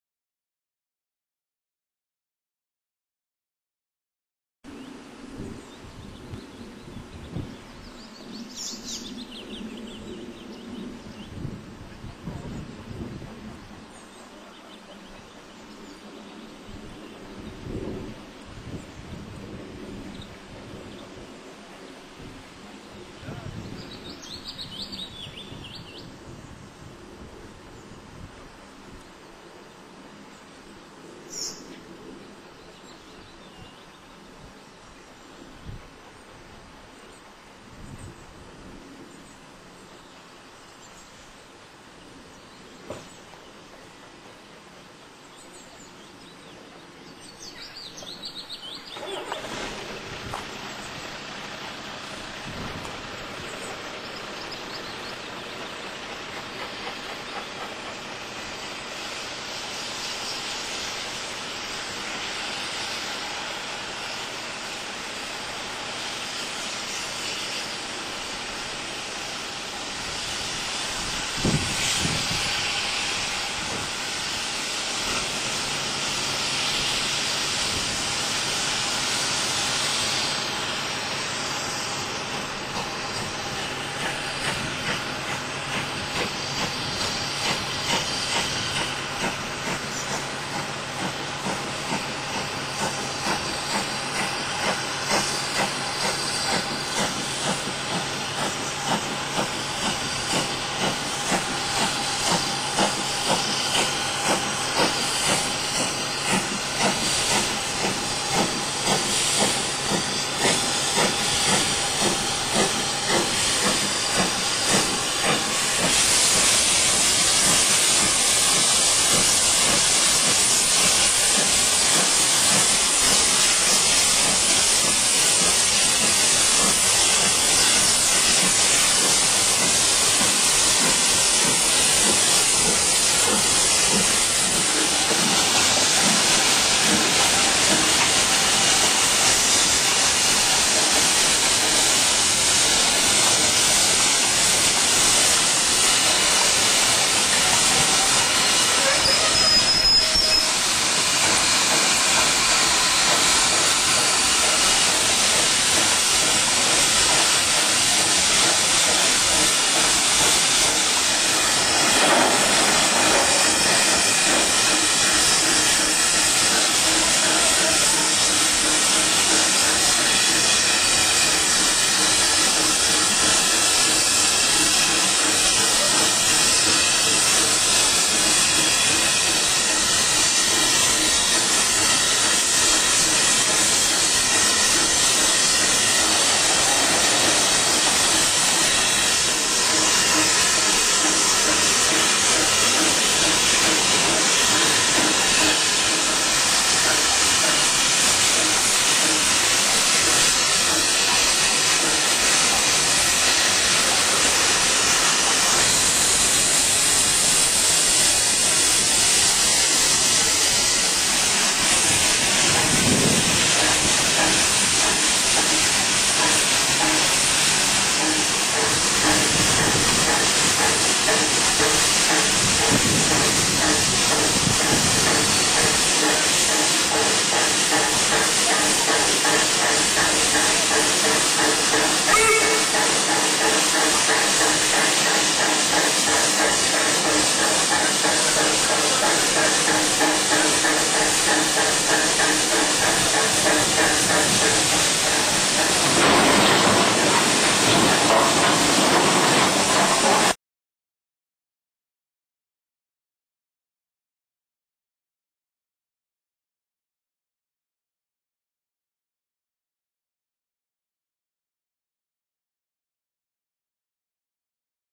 Steam Hauled Railtour At Long Preston
In the summer of 2005 a special rail tour was run from Kings Lynn to Carlisle and return (The Cumbrian Mountain Express). The train was scheduled to stop at Long Preston in North Yorkshire to take on water and coal.